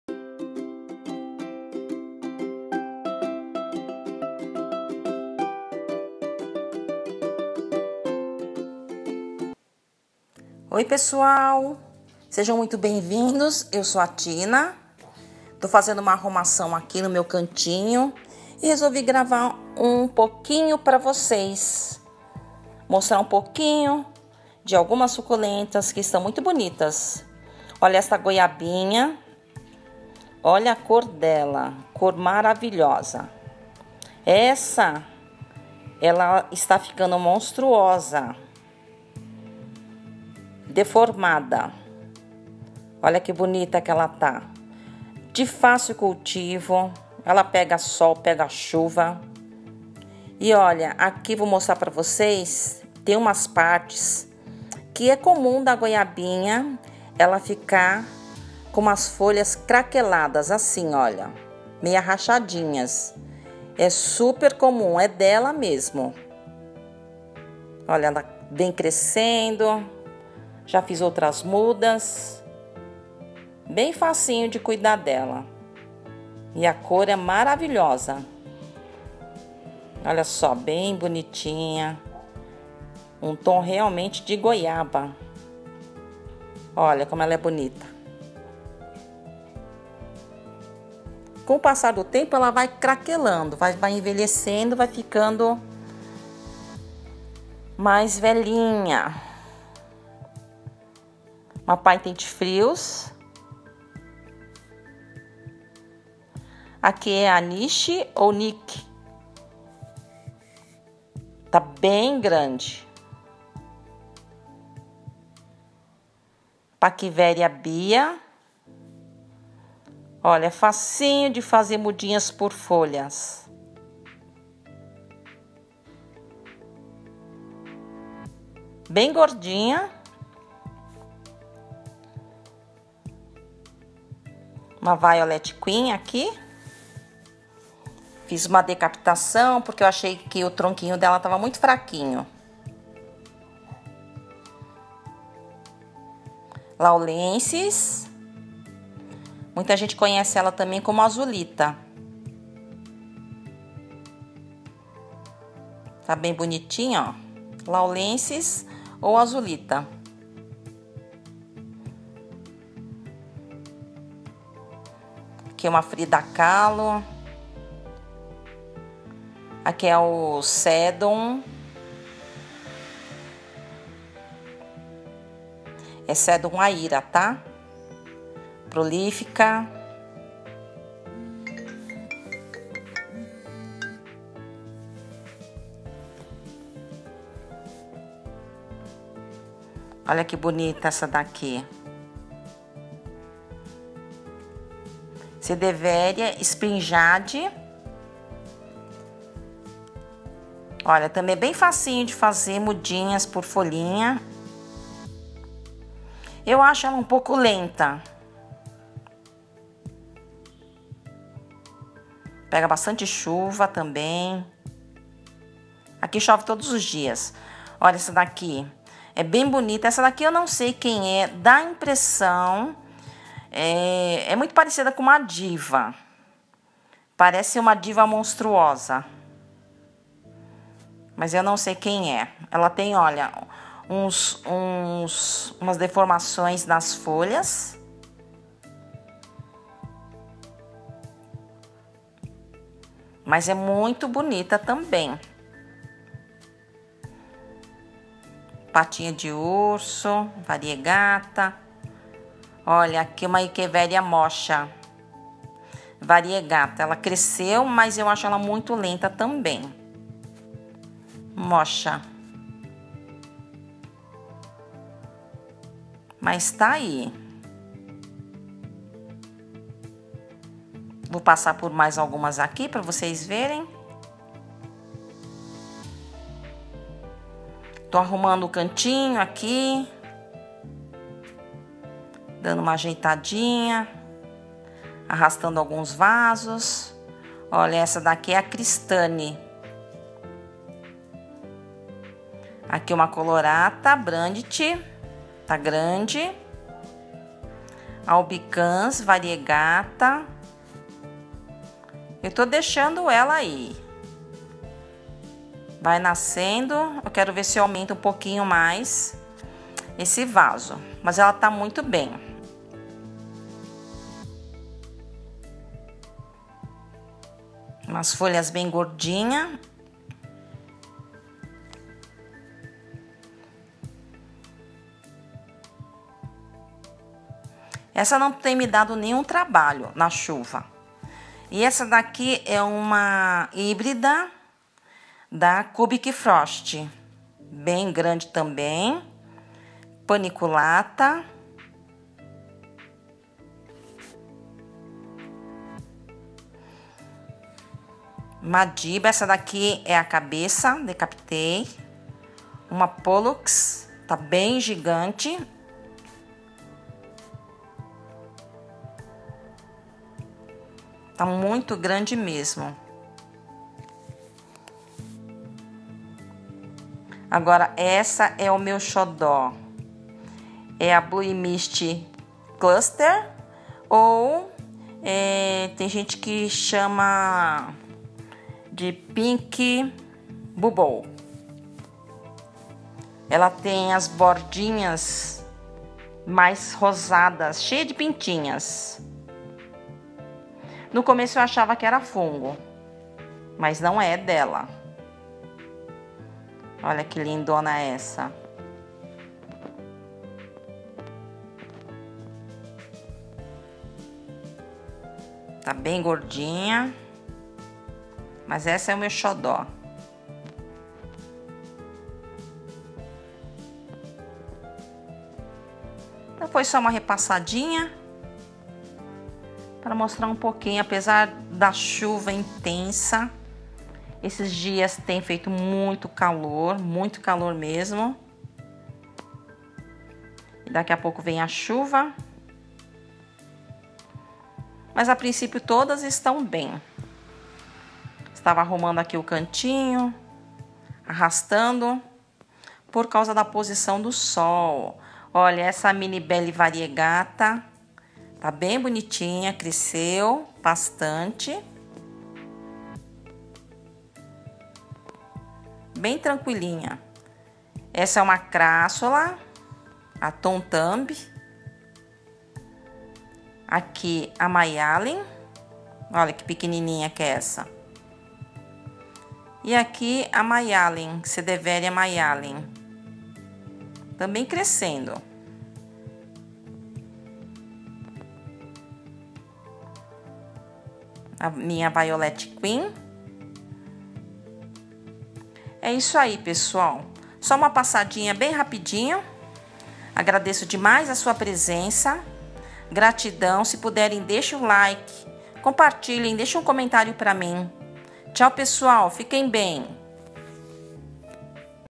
SUCULENTAS DA MINHA COLEÇÃO - ATUALIZAÇÃO
Estou organizando um pouco das minhas suculentas nas bancadas, resolvi atualizar e mostrar algumas

Muitas foram adquiridas no Garden Sacomã, veja na playlist dos Tours no garden